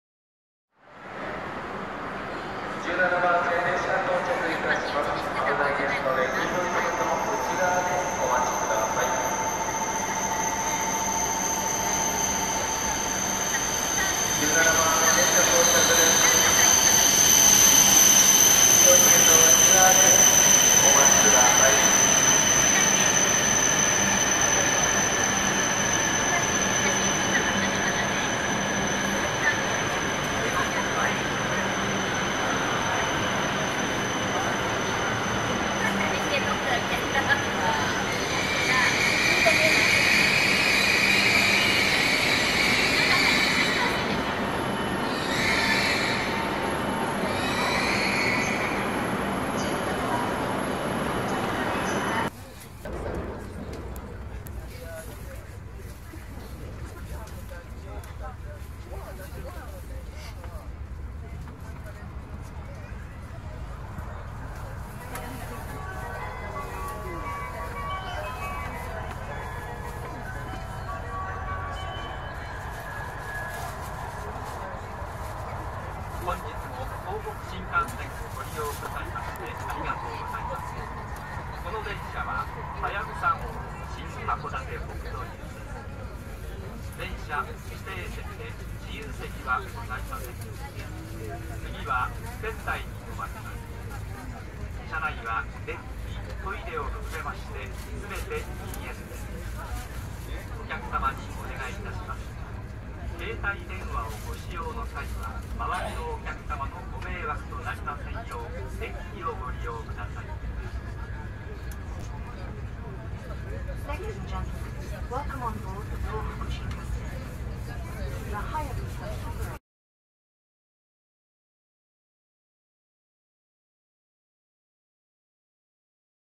東北新幹線・北海道新幹線はやぶさ5号♪新函館北斗行【車内放送】
北海道新幹線へ直通する「はやぶさ5号」

こまち号と併結しない単独編成で新函館北斗へ向かいます。